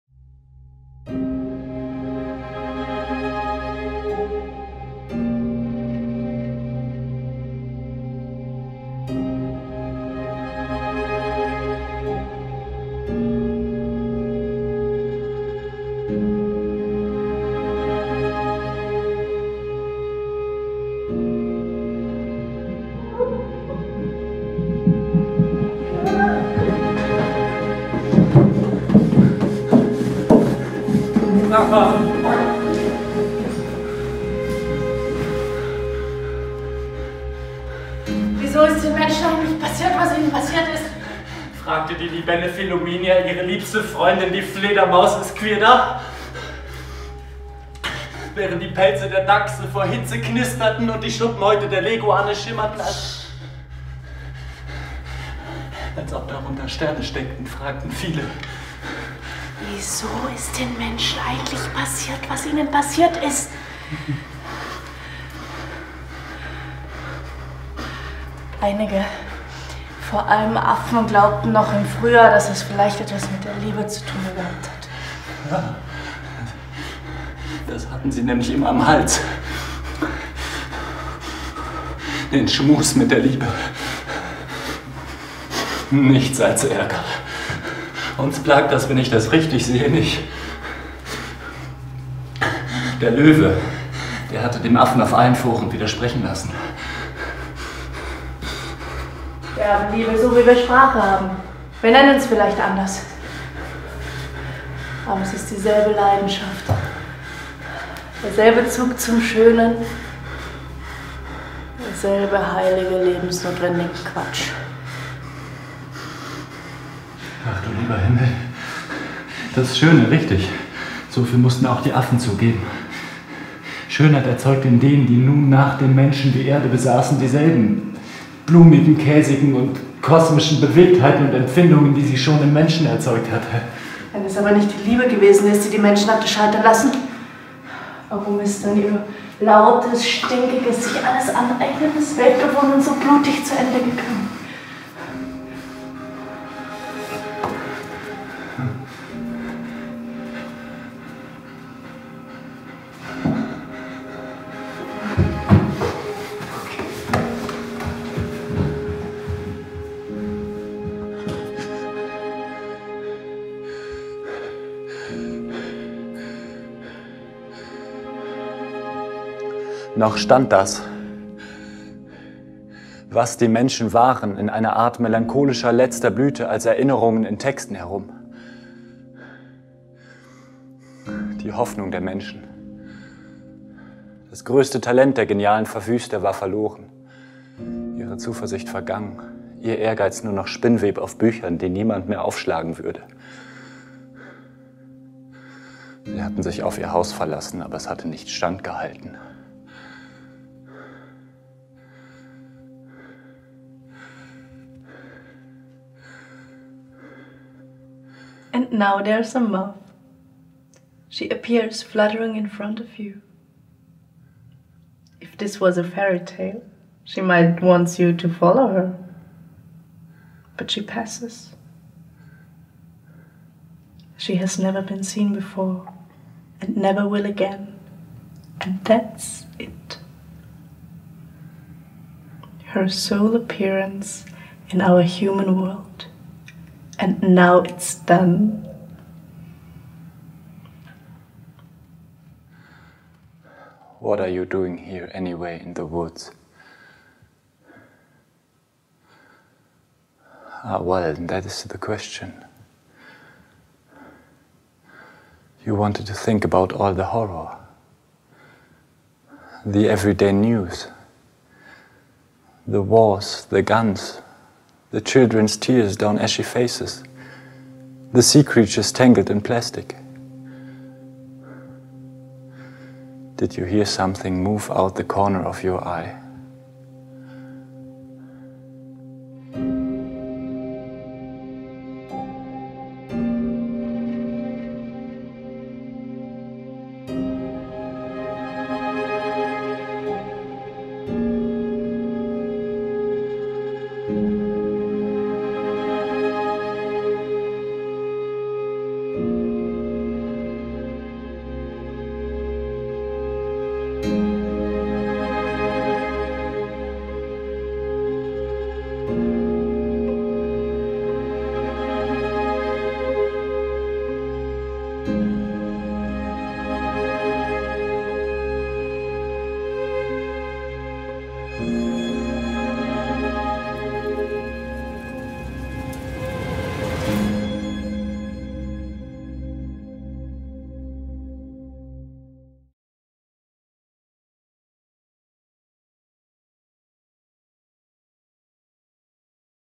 Film trifft Kunst – Was ist Natur? – Museum Sinclair-Haus – Folge 2
Was ist Natur?

Ein literarisches Kurzfilmprojekt in 4 Folgen
Mit Julia Pitsch und Leon Häder
Regie: Kornelius Eich, Komposition: Thomas Osterhoff, Kostüme: Mareike Wehrmann, Videokünstler: Benjamin Lüdtke, Ton: Thomas Joseph

Der Frage „Was ist Natur?“ spüren wir in diesem Projekt mit einer Reihe von vier Kurzfilmen nach. Ausgehend von einzelnen Exponaten und in Verbindung mit literarischen und lyrischen Texten, wird das Thema filmisch betrachtet. Hierbei tauchen unterschiedliche Genres und Stile auf – der Film als Versuchslabor. Wo führt diese Reise hin, wenn Texte von Dietmar Dath, Philip K. Dick, Marlen Haushofer und Silke Scheuermann Denkräume öffnen und weitere Facetten von Naturvorstellungen erkunden?

II. ZUKÜNFTIGE VERGANGENHEIT
Wir schaffen die Menschheit ab und schauen dann zurück. Nur die letzten beiden Exemplare wandeln durch eine Ausstellung einer vergangenen Zeit und fragen sich: Warum ist das laute, stinkige, sich alles aneignende Weltbewohnen der Menschen so blutig zu Ende gegangen? Wieso ist den Menschen passiert, was ihnen passiert ist?

Mit Texten von
Dietmar Dath: Die Abschaffung der Arten
© mit freundlicher Genehmigung des Suhrkamp Verlags, Frankfurt

Kathleen Jamie: Stimme des Waldes  (Surfacing)
© mit freundlicher Genehmigung von Kathleen Jamie??

Mit freundlicher Genehmigung
Carrara Nr. 01, 04, 09, 2005, Fotografien

Was ist Natur?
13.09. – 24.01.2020
museum-sinclair-haus.de

© Museum Sinclair-Haus, Stiftung Nantesbuch. Kunst und Natur, 2020